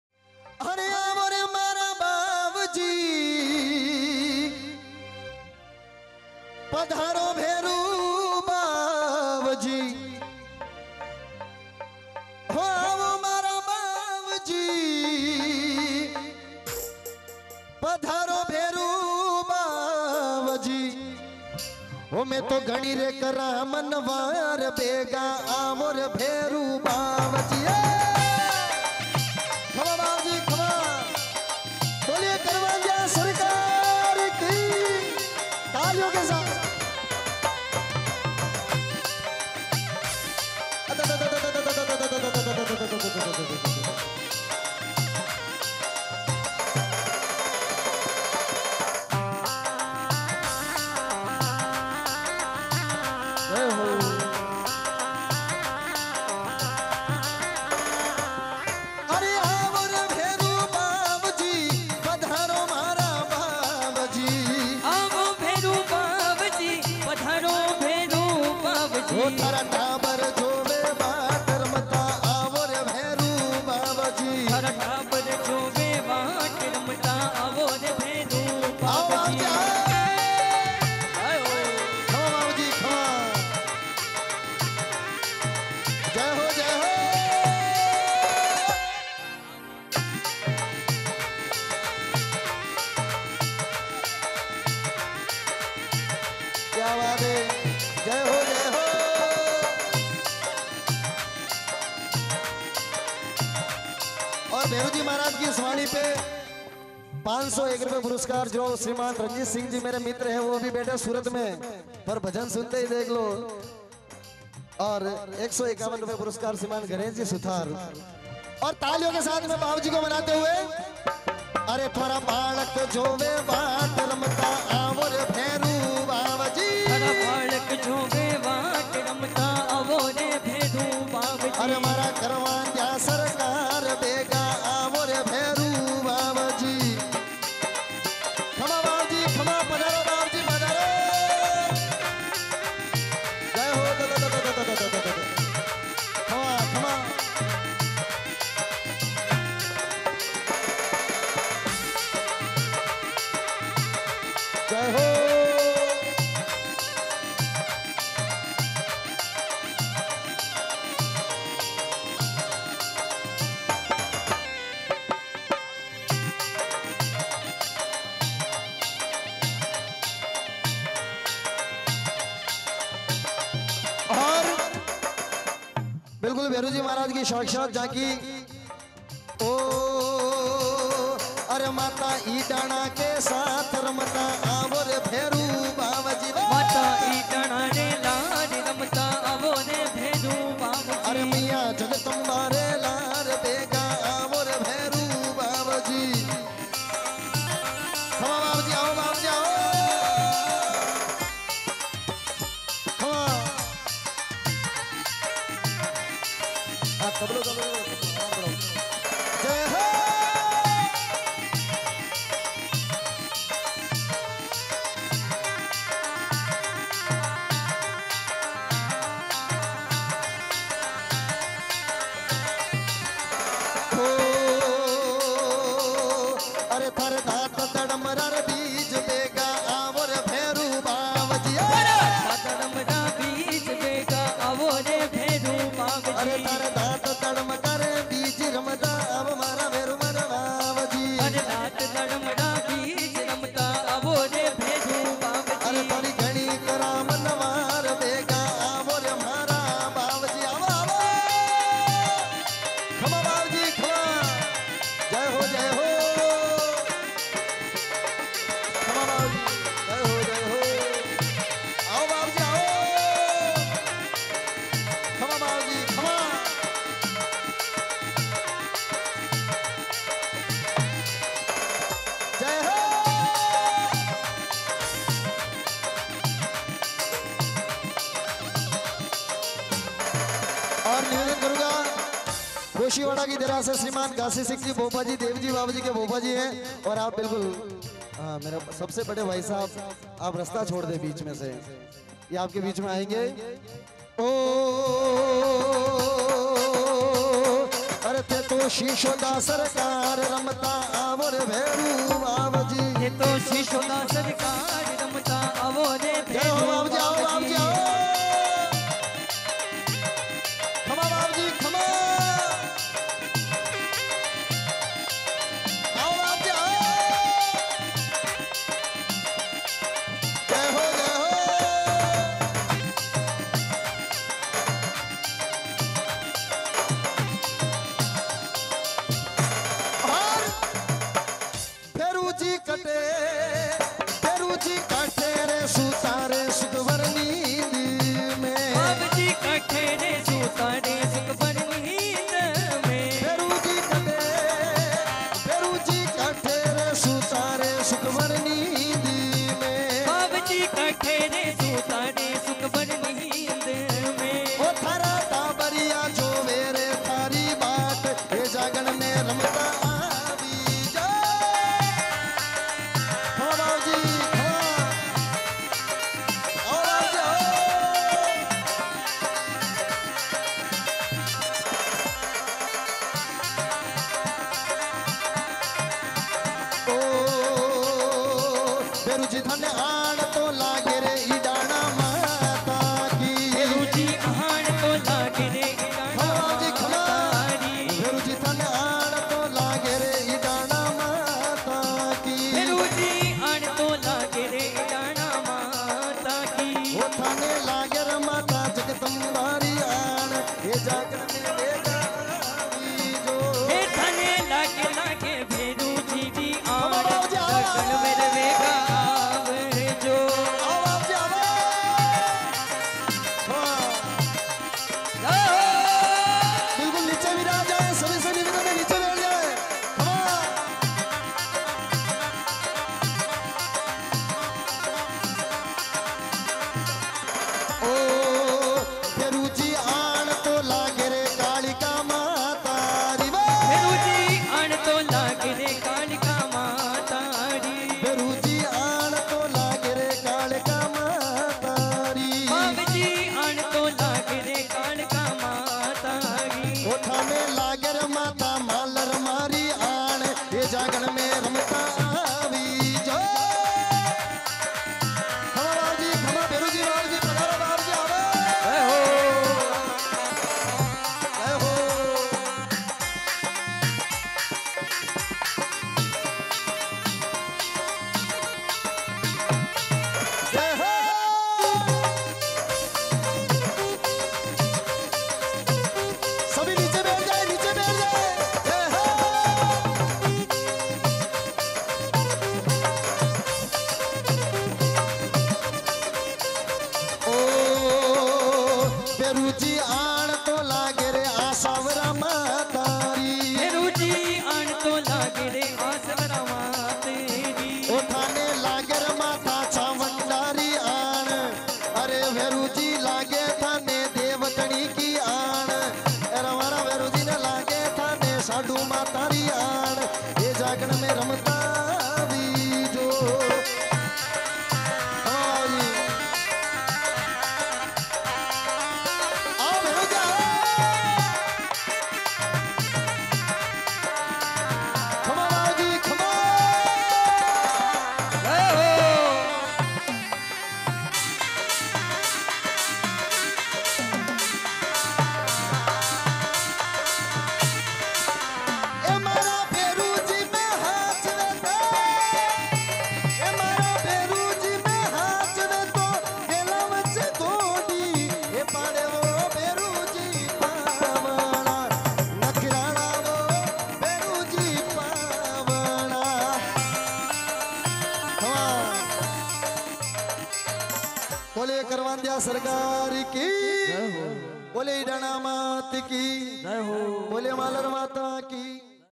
देखिये मुंबई मे भेरूजी का चमत्कार, सैकड़ों भोपाजी को आया भेरूजी का भाव॓ |Bhagvat Suthar |Shivam Live
Bheru ji Bhajan

Singer - Bhagvat Suthar

Mumbai Live

Shivam Studio Gudli Udaipur